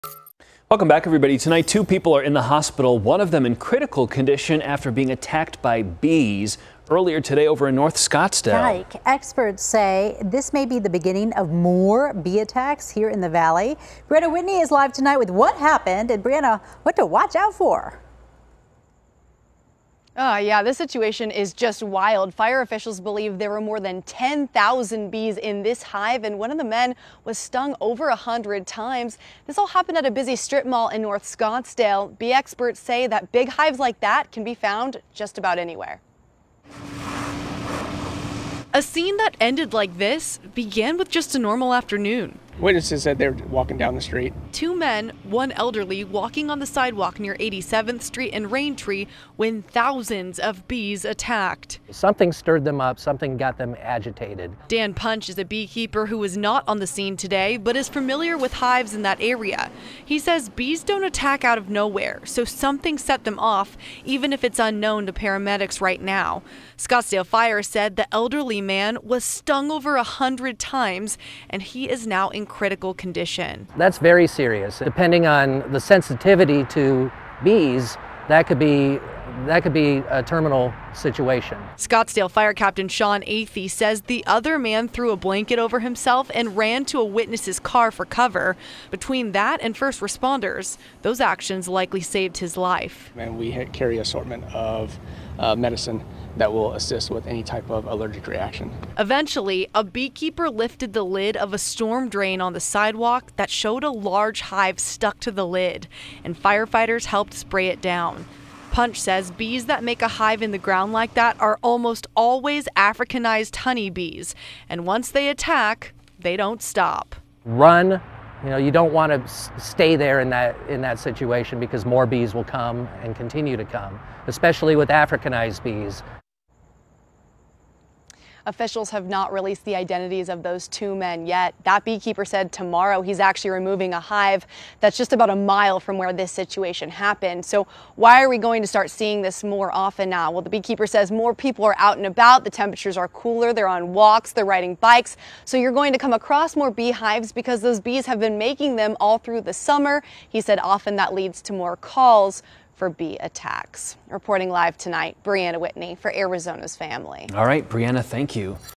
Thousands of bees attack 2 men in Scottsdale
One man was stung more than 100 times and another man was also stung when thousands of bees went on the attack in Scottsdale.